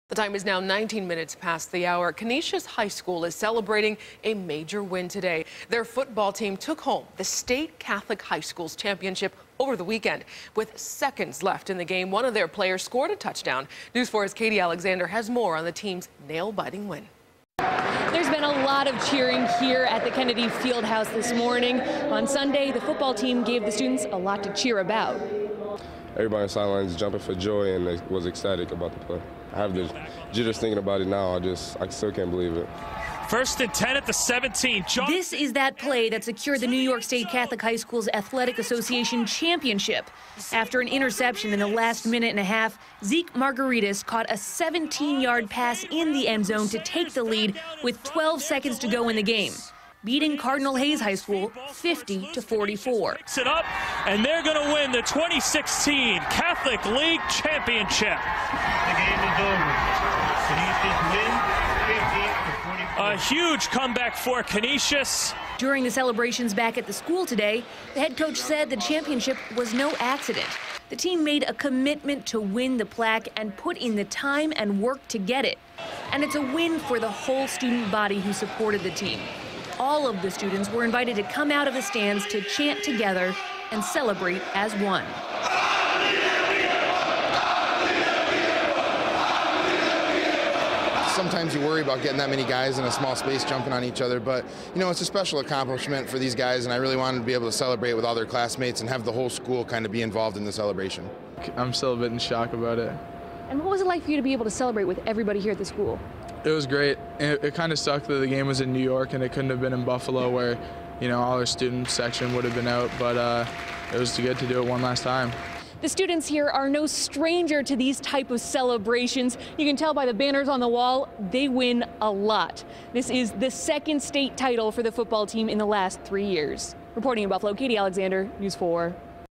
Canisius High School celebrates second state football title in three years
The Crusaders won the New York State Catholic High Schools Athletic Association championship at Fordham in a nail-biter of a game against Cardinal Hayes High School on Sunday in the Bronx.